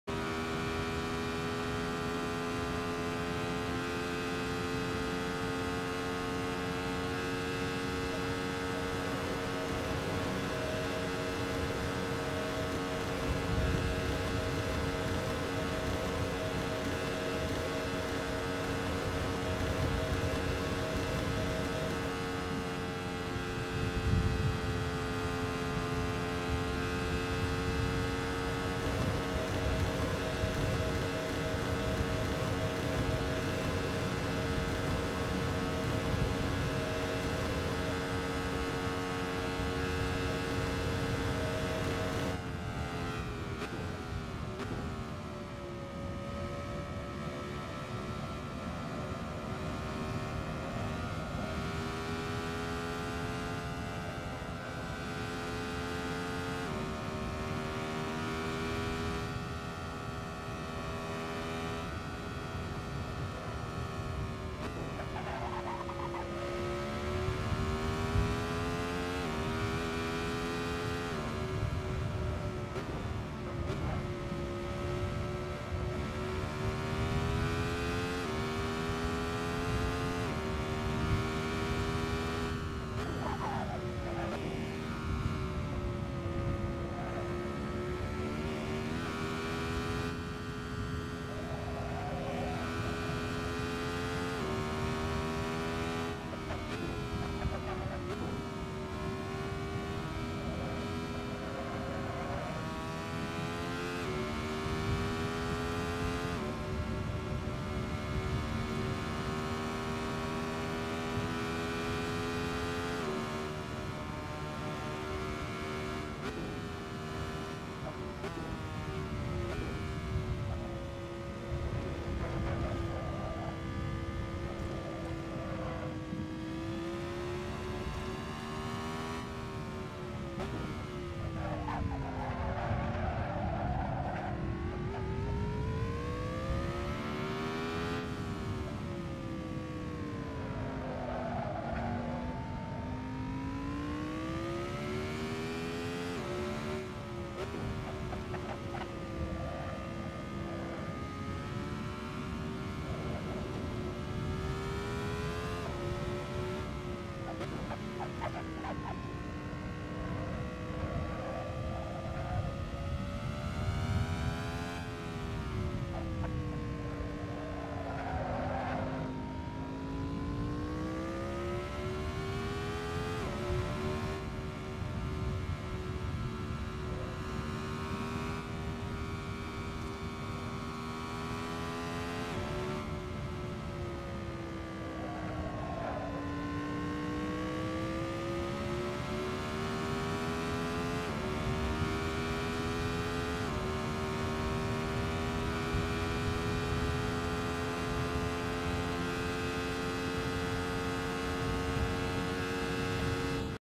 Ariel Atom at the Deutschlandring
Heres a view from the shotgun seat of this alien death machine